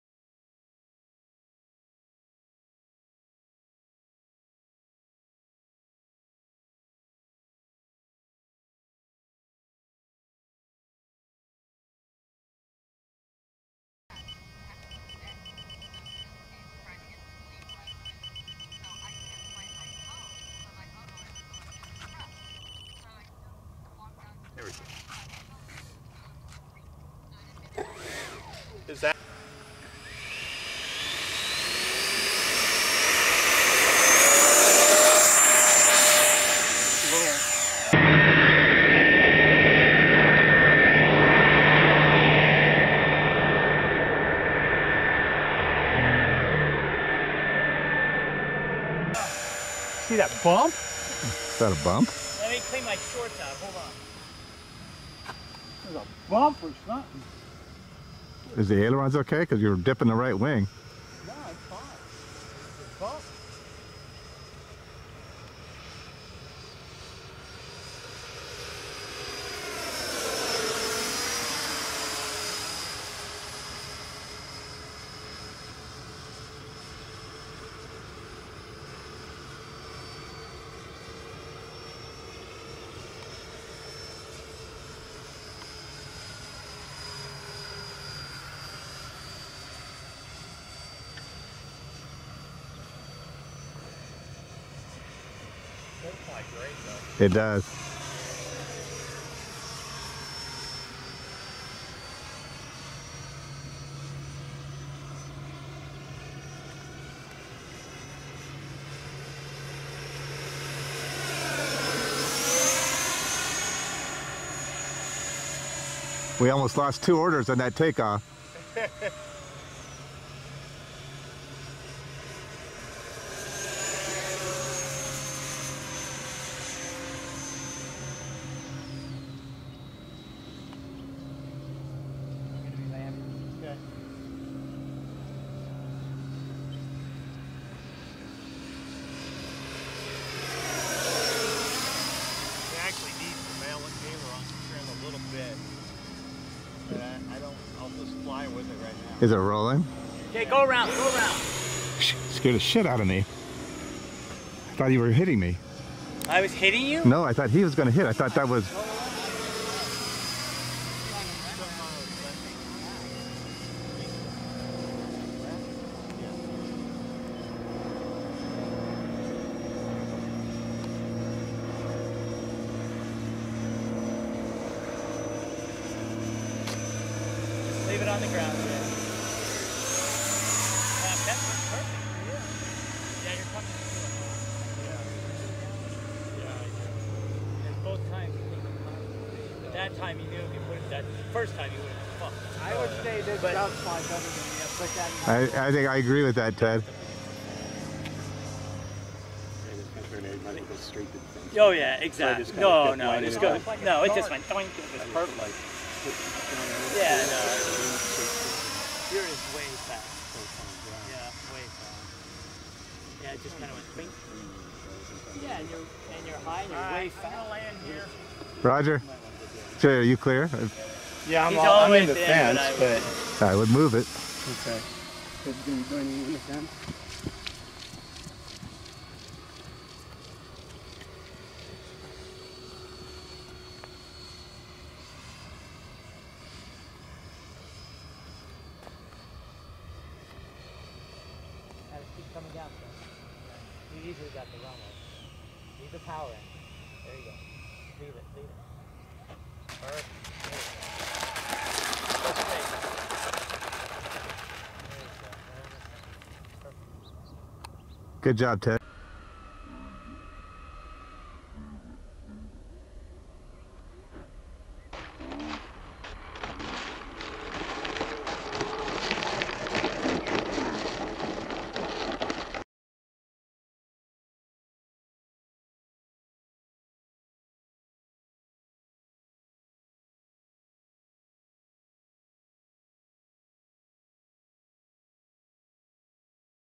CVF WARBIRDS TEDS F/A 18 IOE
Ted's first flight with the new FMS F/A 18 Super Hornet.